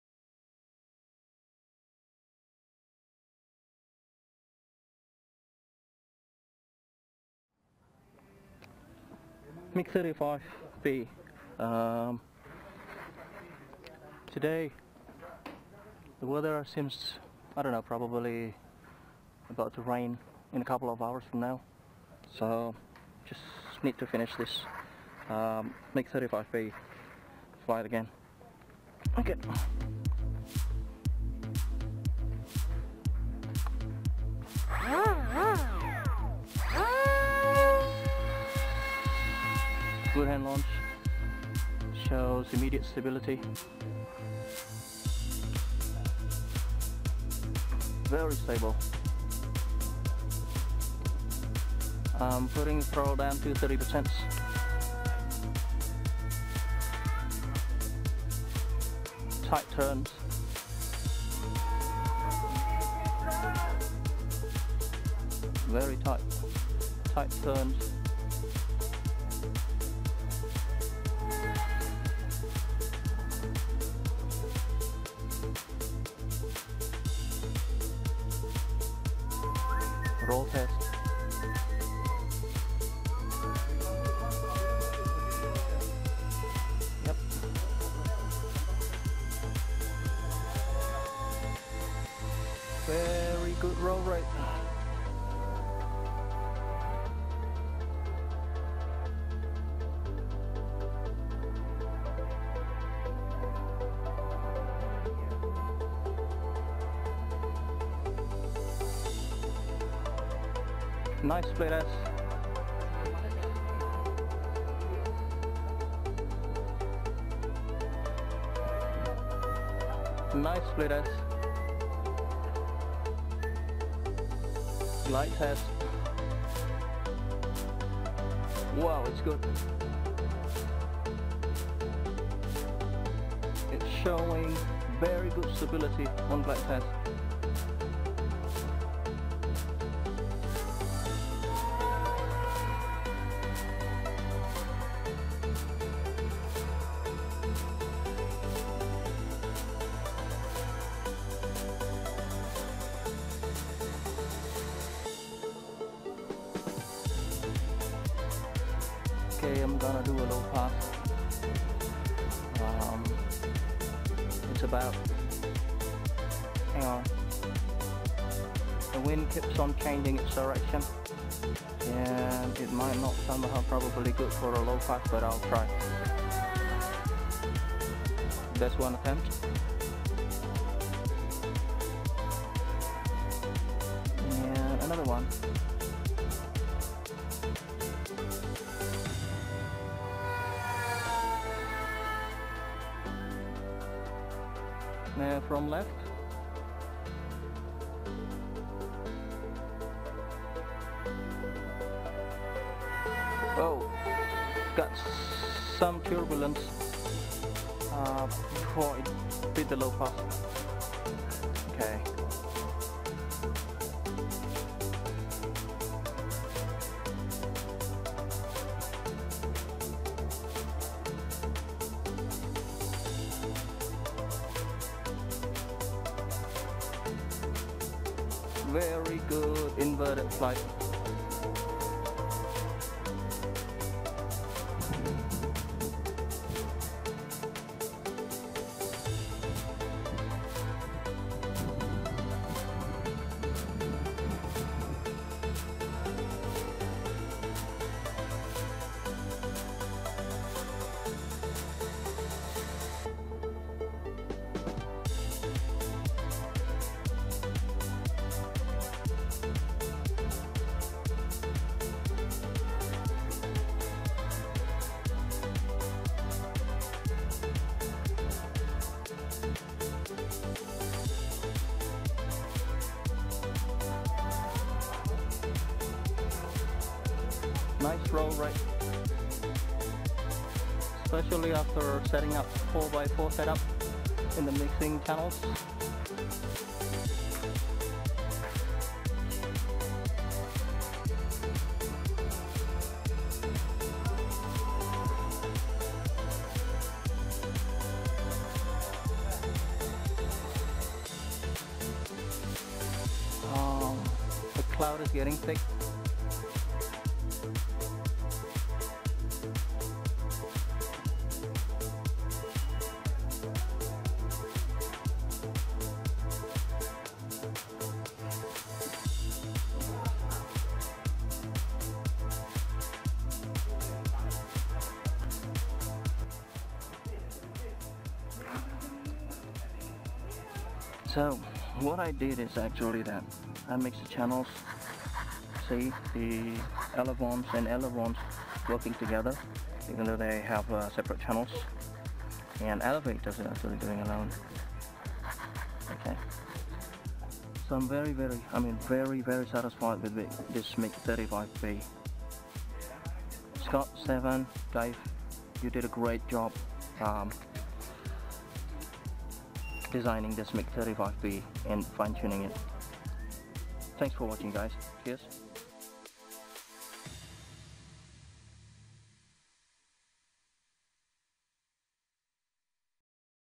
NAMC Mig-35B Fulcrum F Flight Tests
This is a post maiden video, taken to observe the Mig-35B's flight envelope.

This RC Scratchbuilt is setup to have ailerons and elevons mixings without rudder. It delivers solid roll rate and high responsiveness.

Some manuvers showed high stability and well balanced airframe, with soft touch landing in the end.

NAMC, you guys have done amazing work in designing and fine tuning this parkjet. It's now officially replacing my decommissioned F-18V3 FPV, forward.

Welldone, guys!